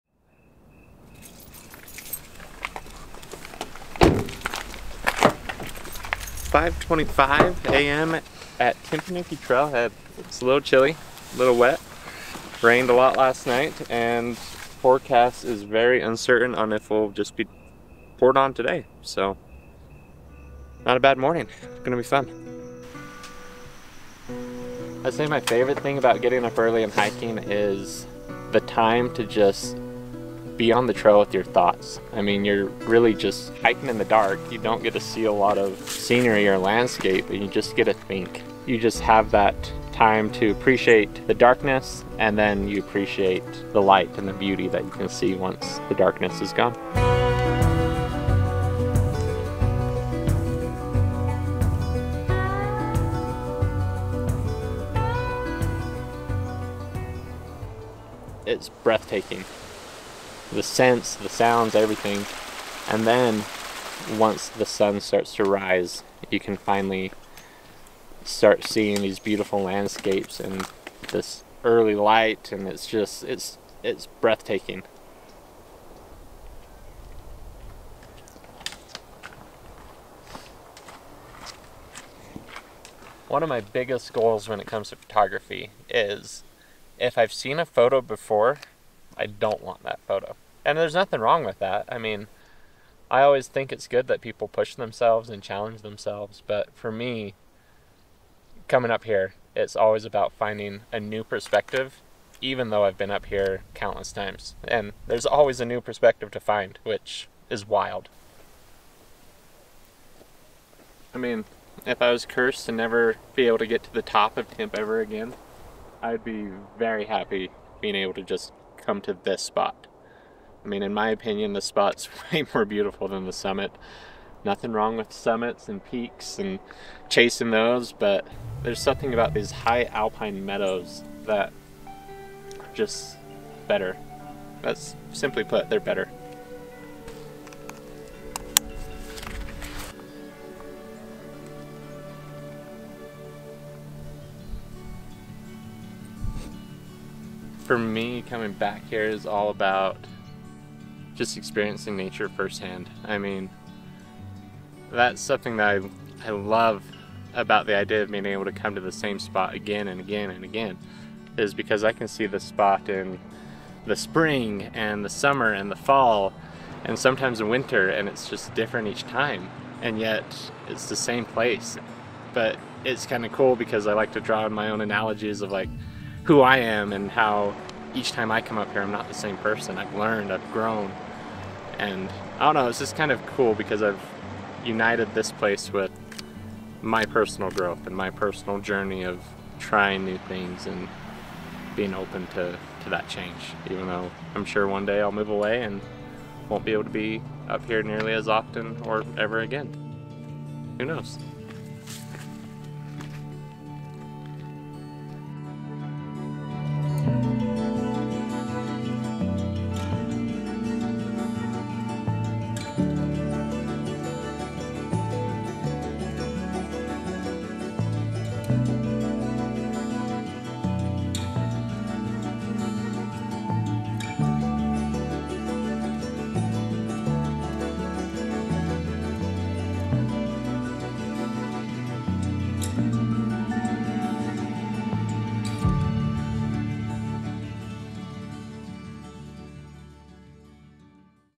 The Basin - A Short Doc with Wyatt Peterson
Wyatt Peterson is a passionate adventurer and photographer based in the state of Utah.  He has frequented the upper basin of Mount Timpanogos dozens of times, including 16 times in 16 weeks during the summer of 2022.  In this video he chats about why he continues to return to this location, and what it means to him.